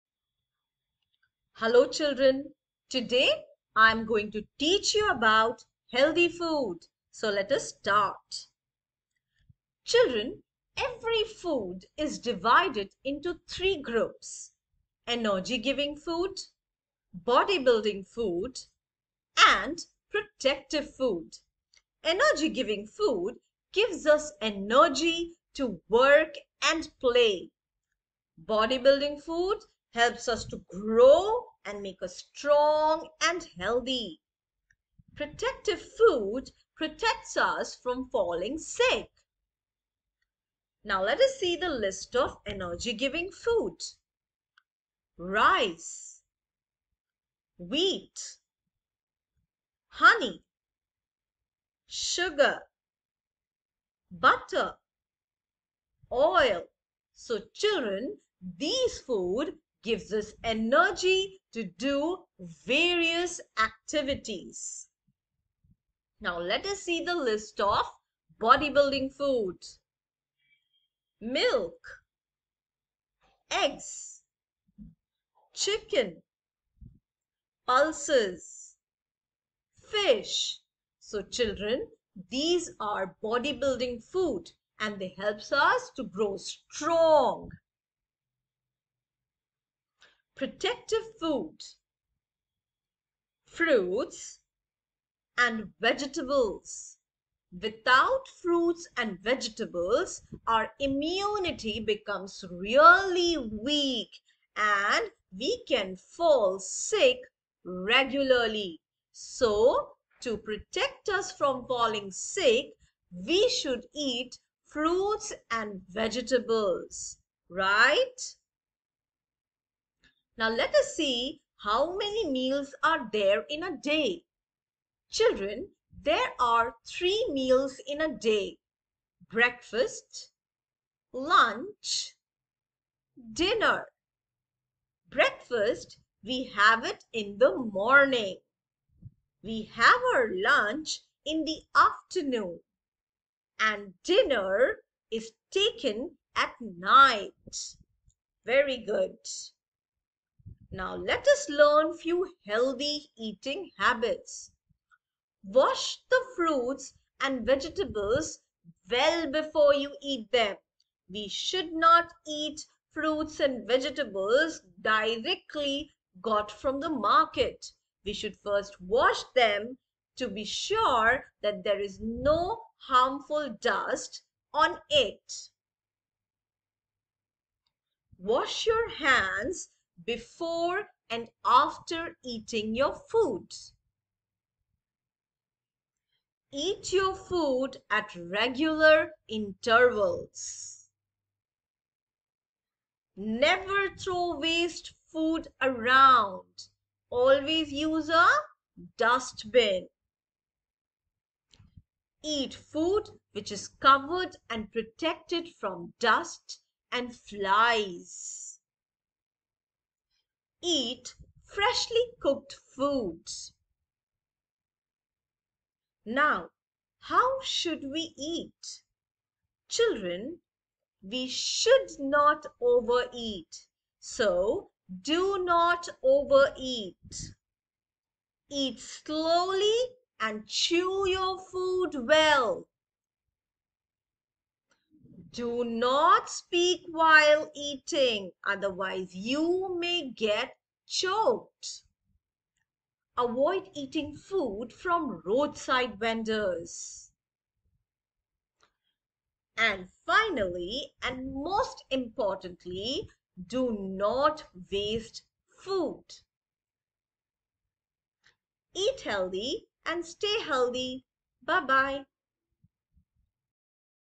Healthy Food EVS Class 2 | Eating Healthy | Healthy Eating Habits | Science Grade 2 | CBSE Class 2
This video will help your child to understand the food groups with examples, meals in a day and, healthy eating habits.